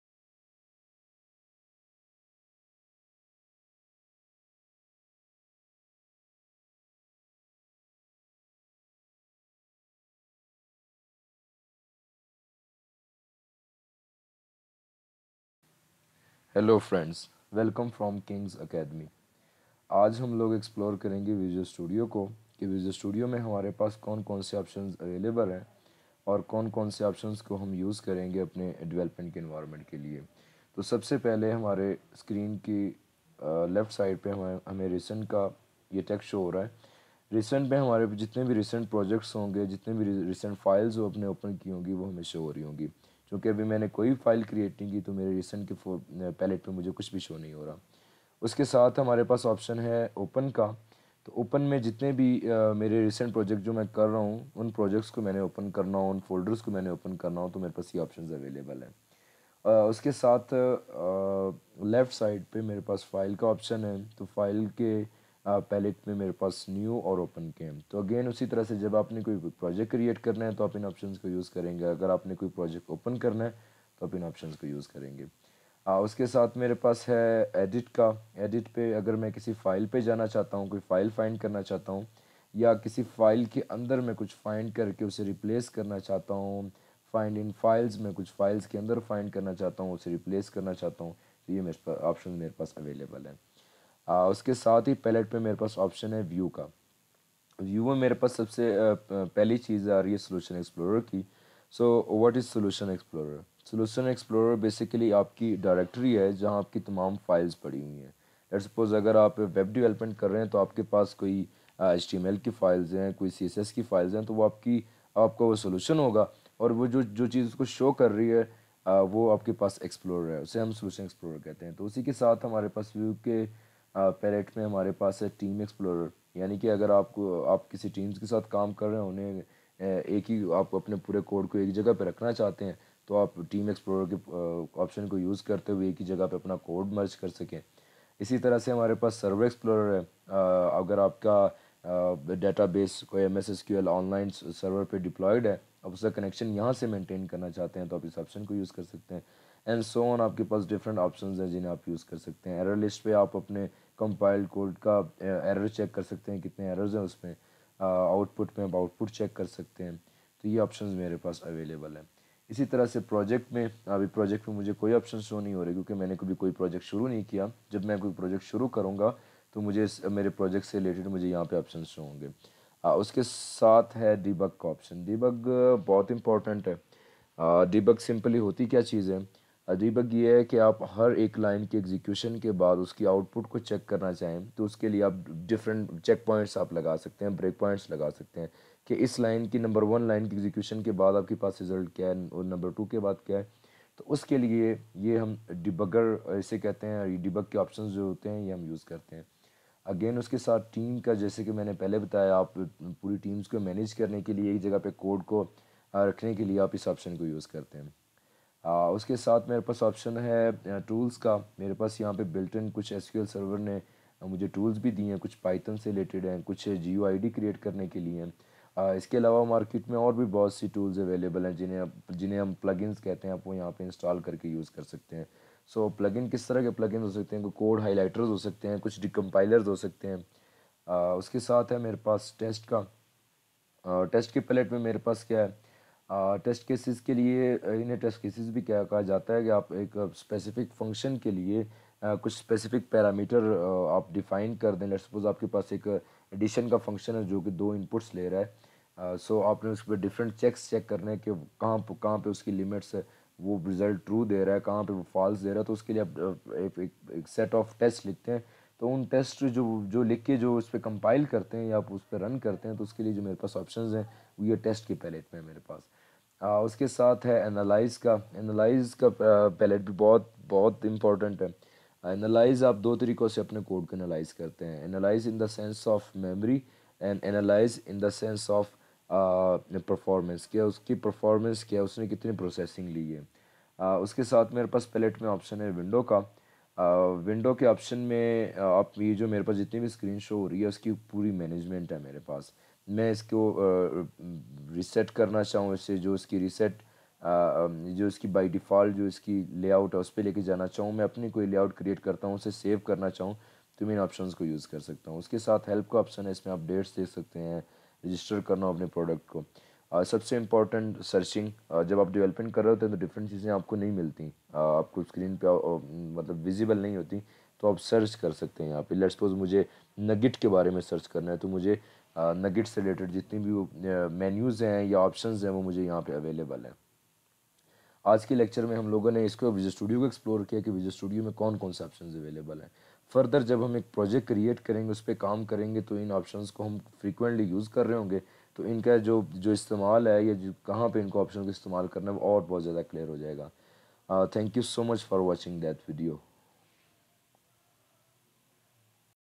Introduction to Menu Bar - Urdu | Visual Studio | 01
This video tells you about the Menu Bar of Visual Studio 2017 - Community Version by covering basics of following topics:
- File Creating Option
- File Opening Option
- Team Explorer
- Solution Explorer
- SQL Explorer
- Debug
- Performance Analyzer Option
- Tools & Plugin Option
- Setting Windows Layout
- Searching Option